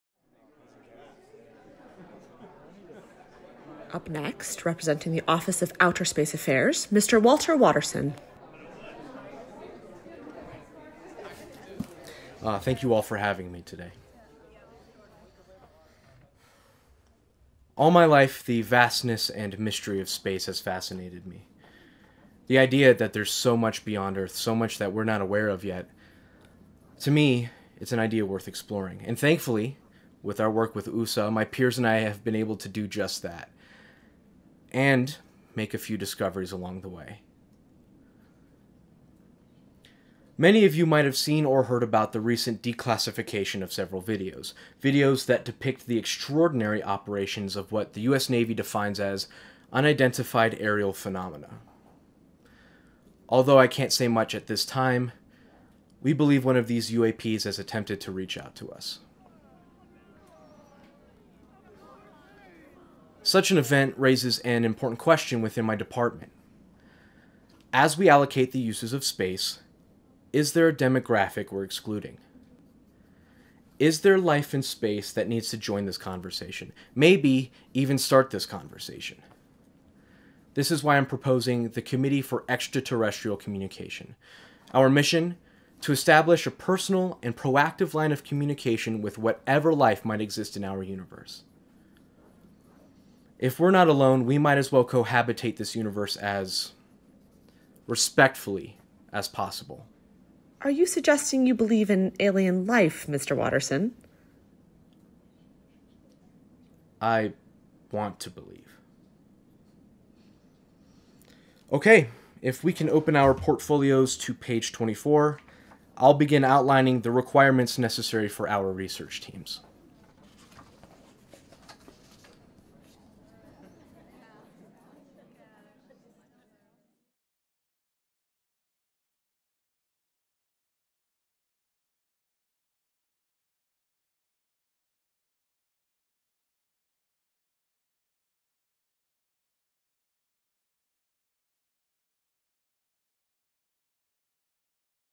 "The Committee for Extraterrestrial Communication" | The Monologue Series
The existence of 'alien life' was such a taboo topic for many years, until sightings of unexplainable aircrafts increased drastically. First leaked to the public, videos captured by US Navy fighter jets between 2004 and 2015 depicted what they came to refer to as 'Unidentified Aerial Phenomena'. Walter Watterson (played by Tobias Kacsandy) is an employee for the United Nations Office for Outer Space Affairs with an affinity for all things extraterrestrial, and he has a feeling the UAP sightings are a sign for things to come. At the UN Committee Summit, he decides to roll out his latest project - The Committee for Extraterrestrial Communication.

Monologue Written and Performed by Tobias Kacsandy
'Panel Leader' played by Grace Wedgwood